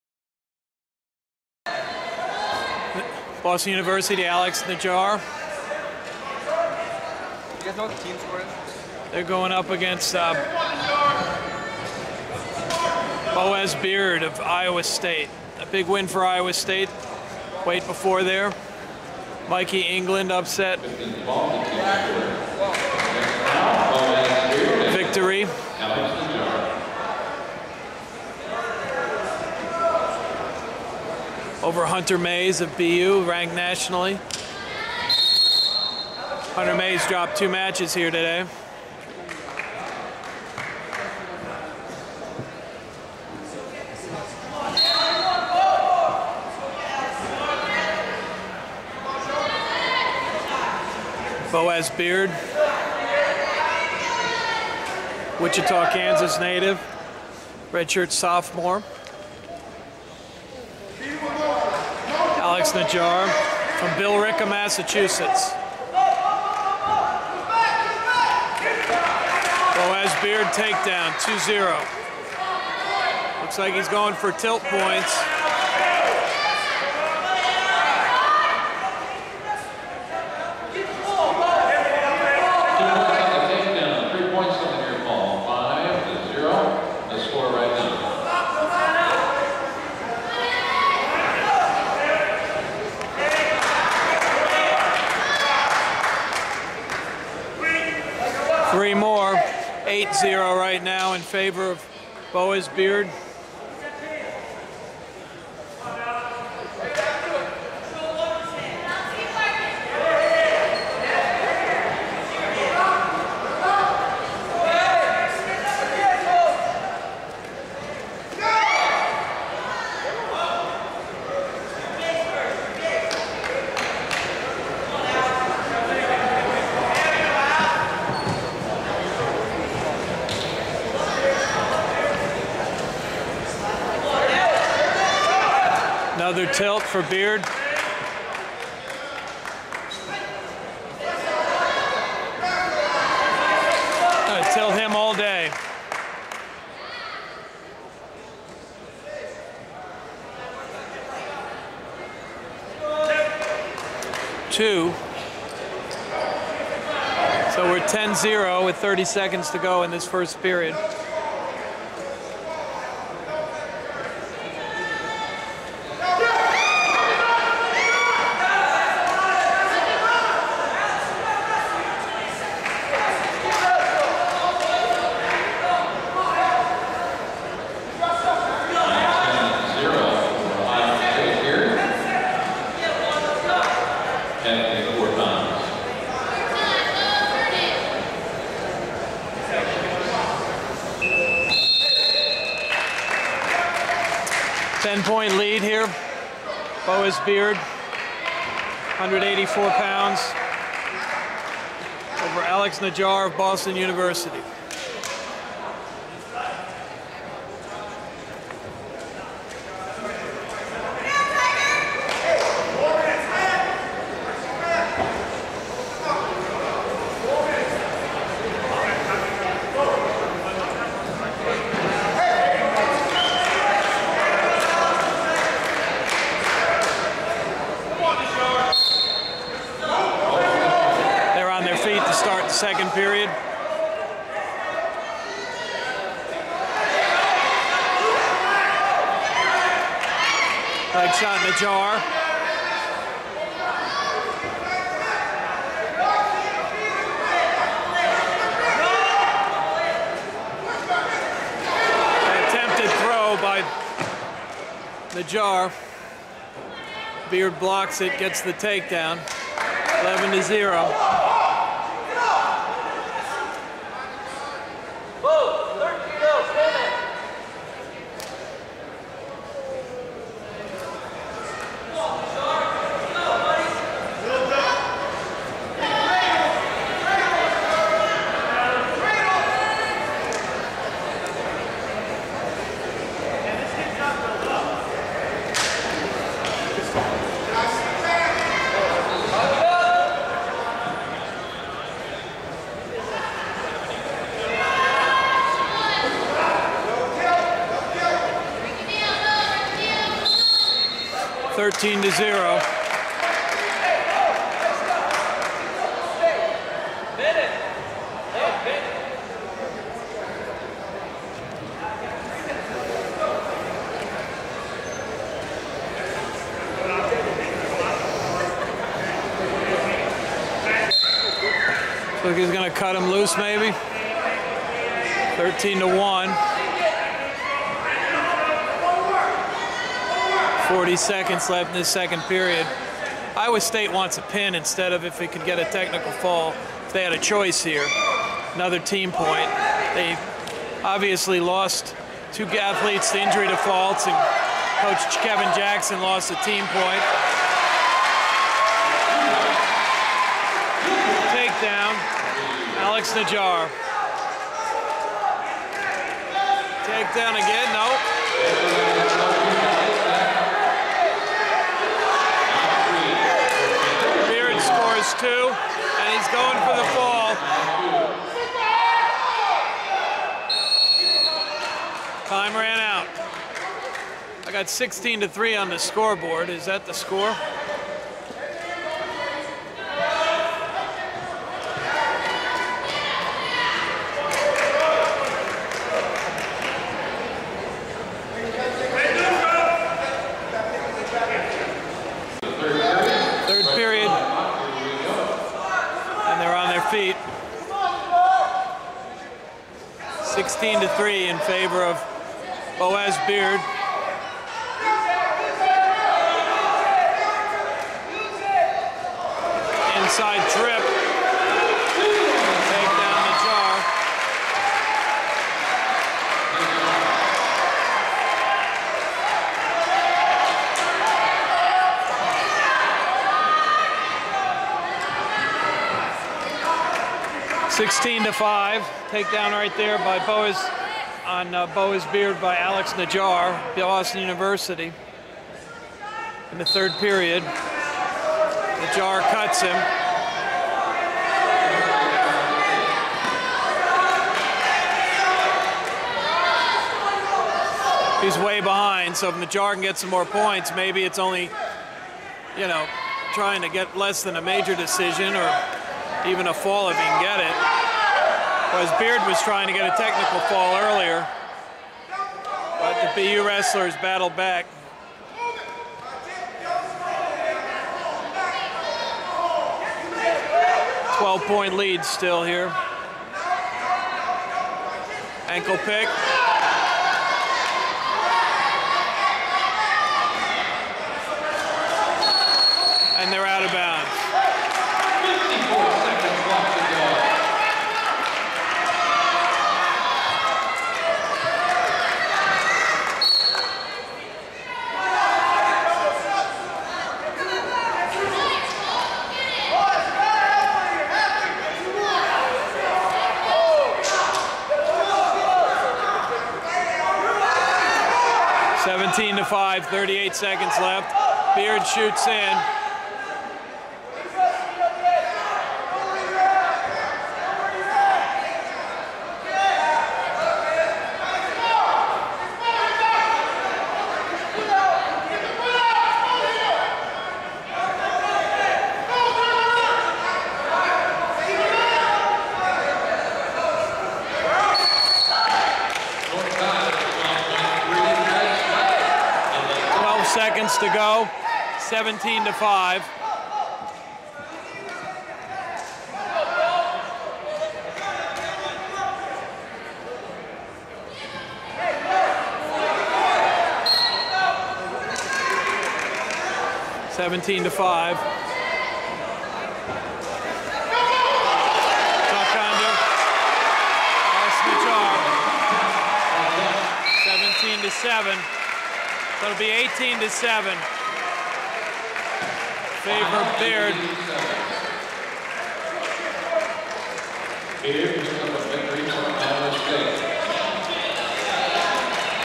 Boaz Beard (ISU) major dec. Alex Najjar (BU), 18-7 at 184 lbs.
Boaz Beard (Iowa State) major dec. Alex Najjar (Boston), 18-7 at 184 lbs. in dual meet on Nov. 6, 2011